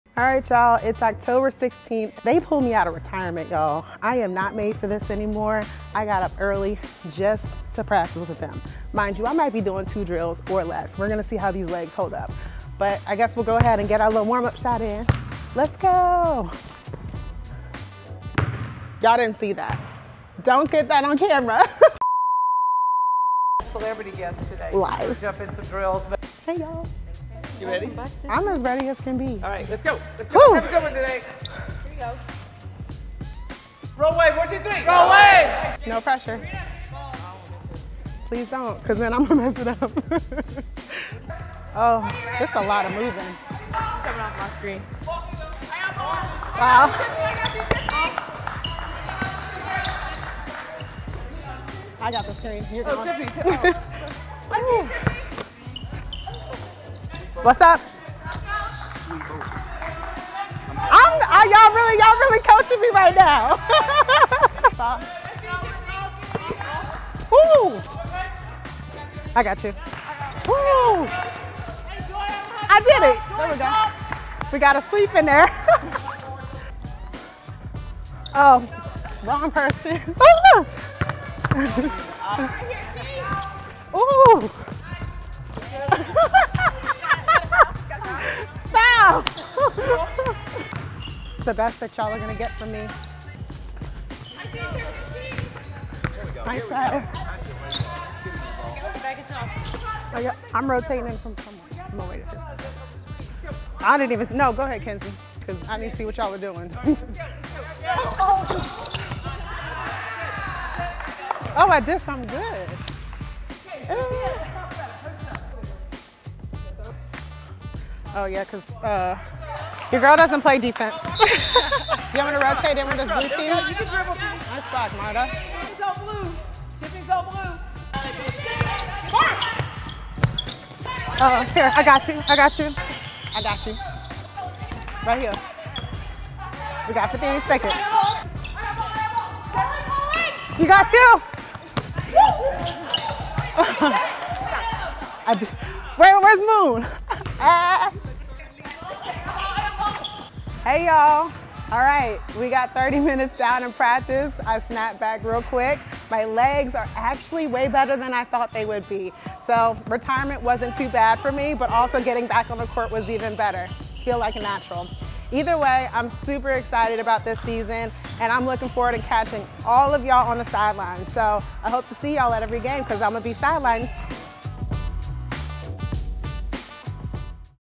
Tulane Women's Basketball: ESPN+ Analyst Takes On Practice
ESPN+ Analyst and former Tulane Women's Basketball player, Tiffany Aidoo, came out of retirement and jumped in on a practice.